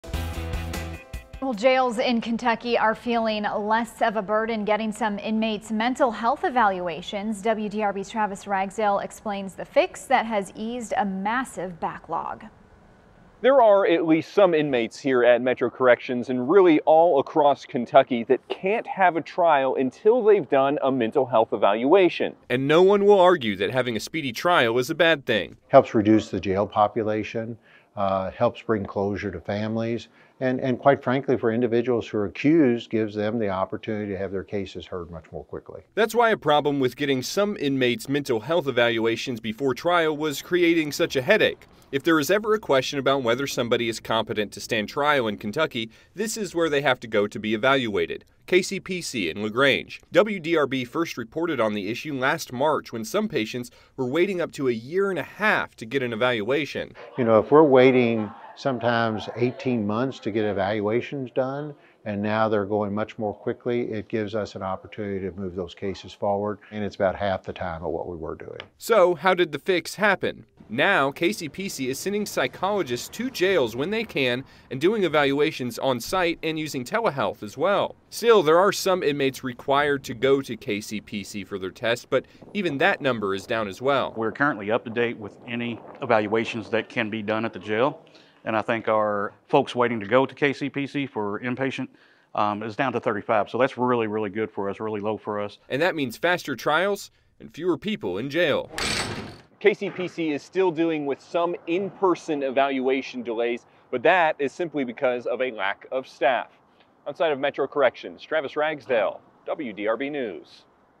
Year or more wait for Louisville defendants needing competency evaluations before trial has been cut
As recently as this past summer, the wait for a defendant in Louisville to get a competency evaluation at Kentucky’s state-run psychiatric center was a year or more, leaving people jailed indefinitely and victims unsure when they would get any resolution.

The facility can only evaluate about 30 defendants at a time, as compared to 78 when fully-staffed, she said at the time.